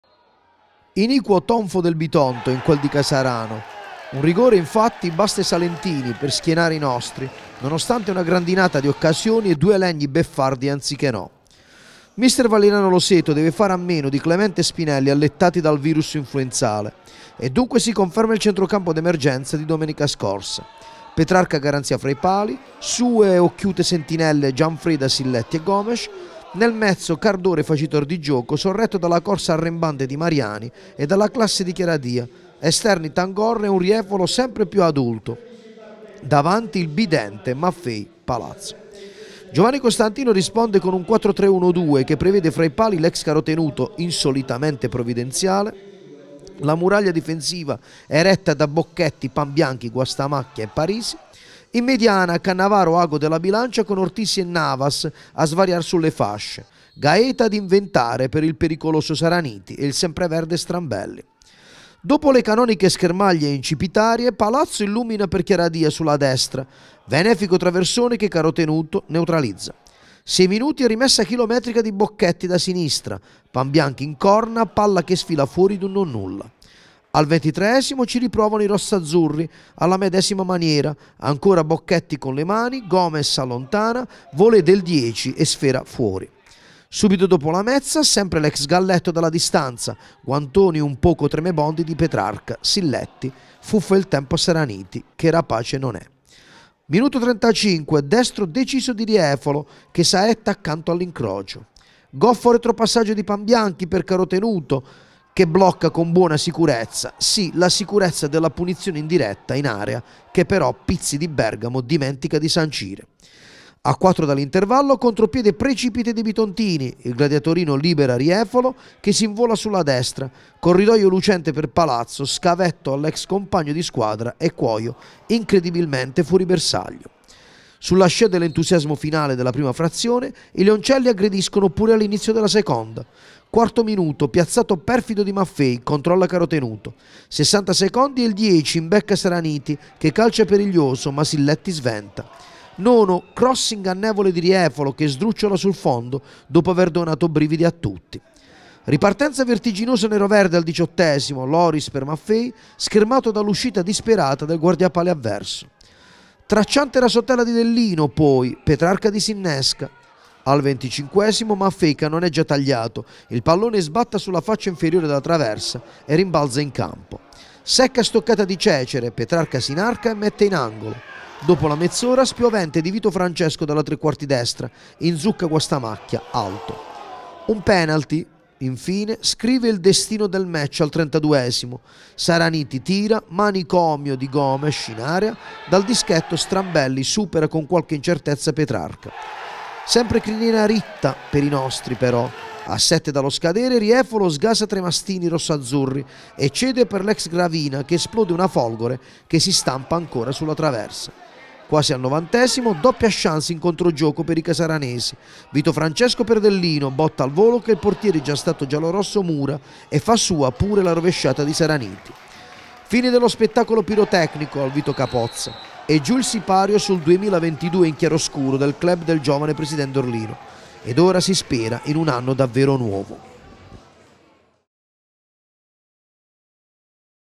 Il Bitonto crea e spreca. Il Casarano lo punisce e vince di rigore
Casarano-Bitonto 1-0, gli highlights del match